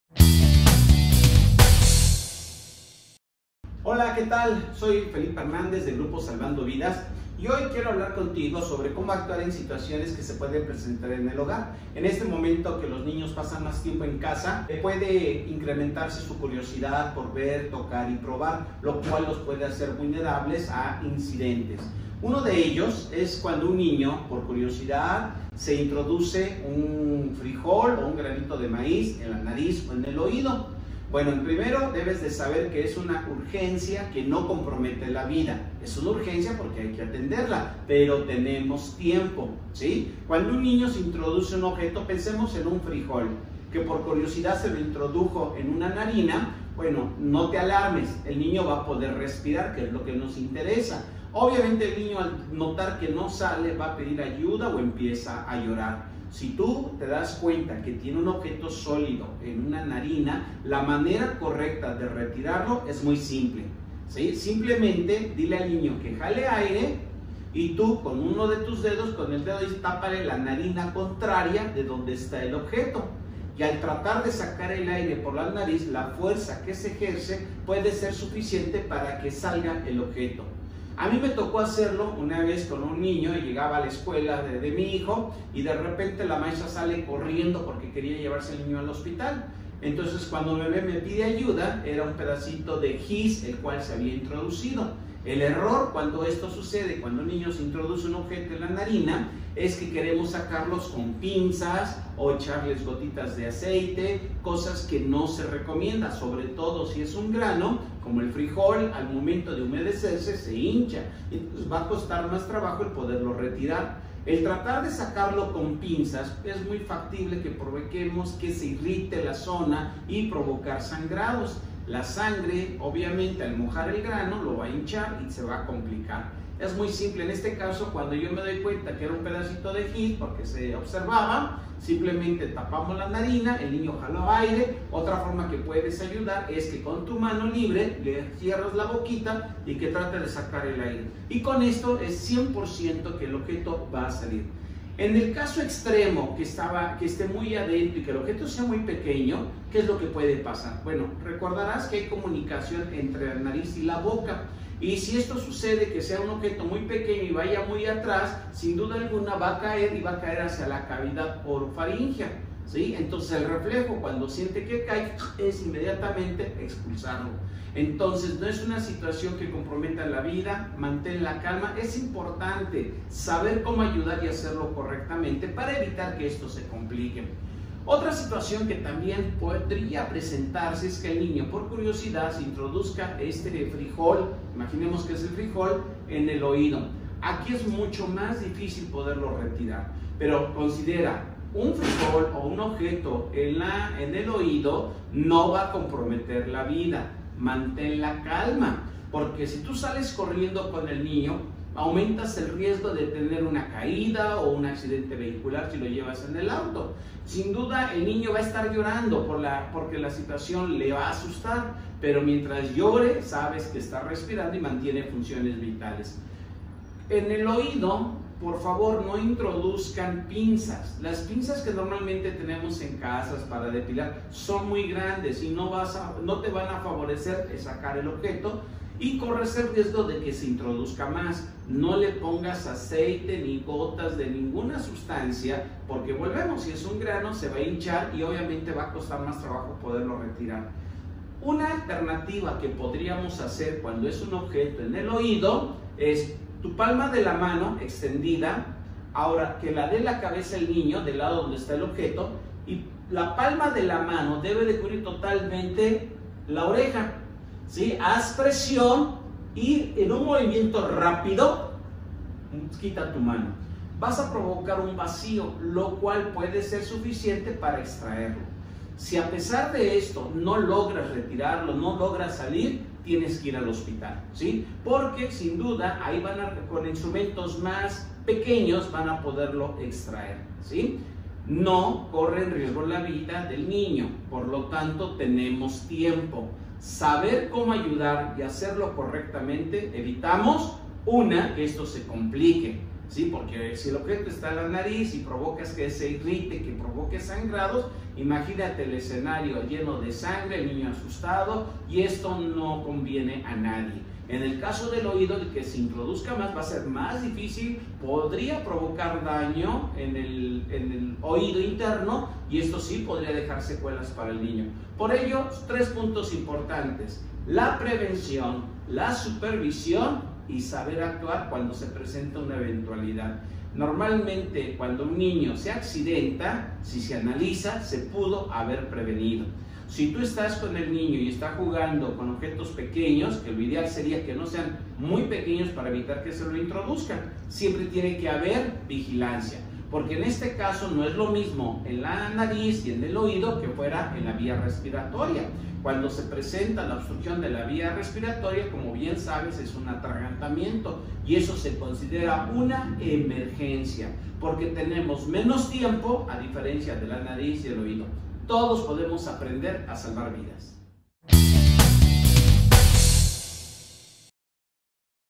Objetos nariz y oído
El asistir una urgencia por objeto extraño alojado en nariz u oídos, no debe ser traumante o completamente desesperante.